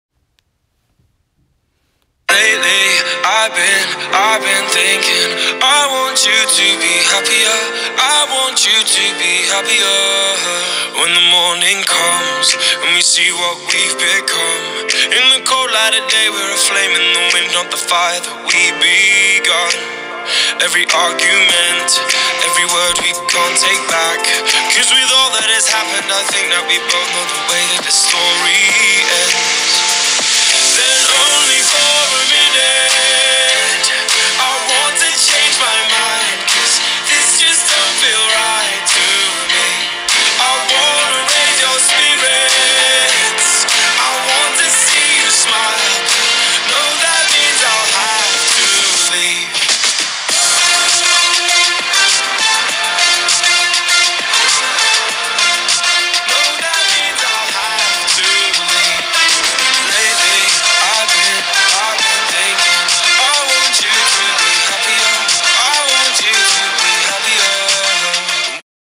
NHS Color My World Color Run 2018// Mental Health Awareness and Teen Suicide Prevention
Thank you to everyone who made my dream become possible 🌈❤️